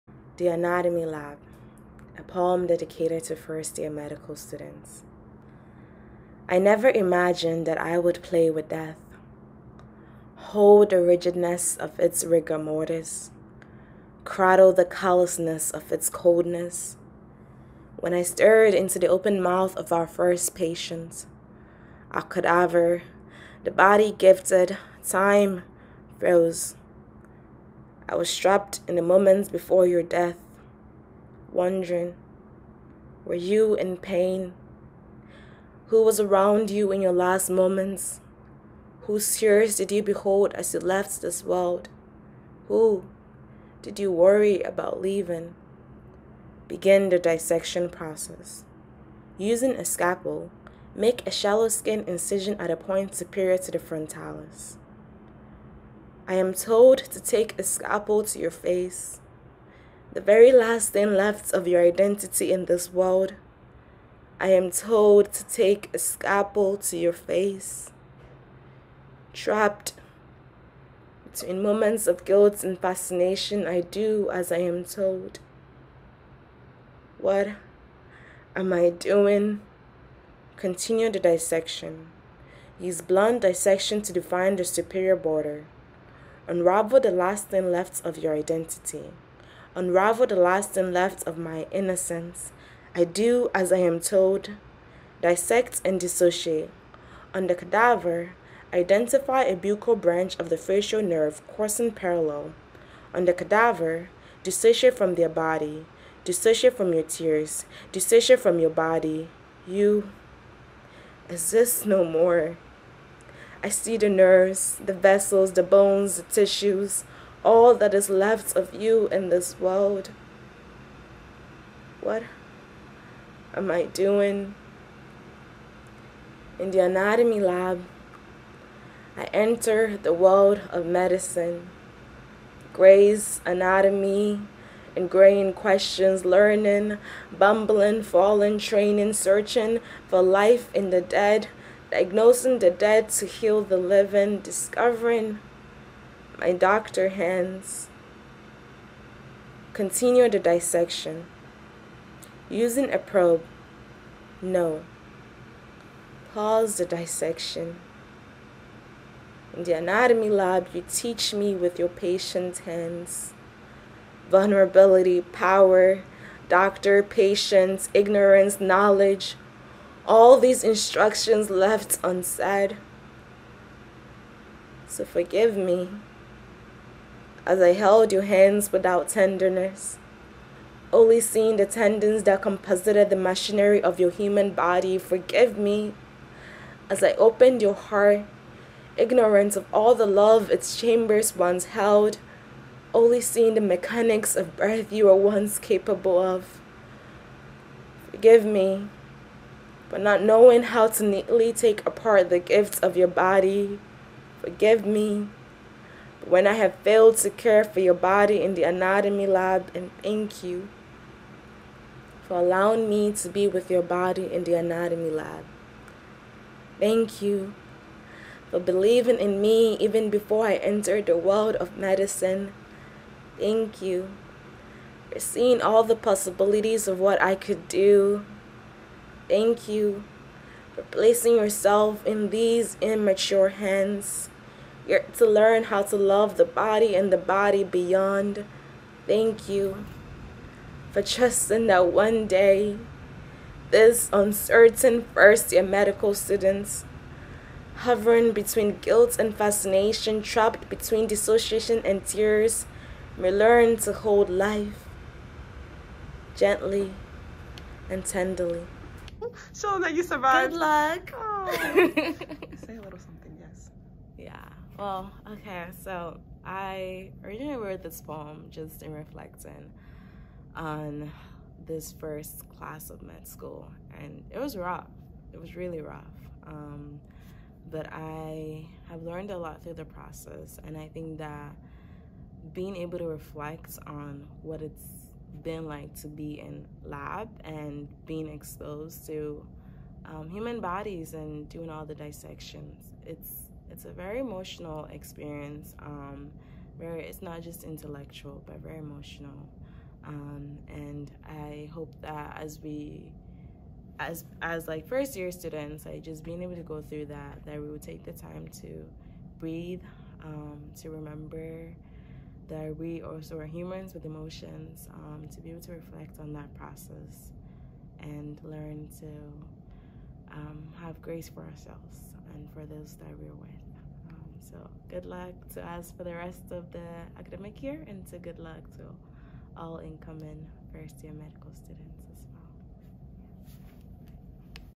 The Anatomy Lab: A Poem Dedicated to First Year Students
An aspiring physician-poet, who through poetry, is learning to see the world as stained glass: broken yet beautiful. Listen here to my reflections on the  first class in medical school.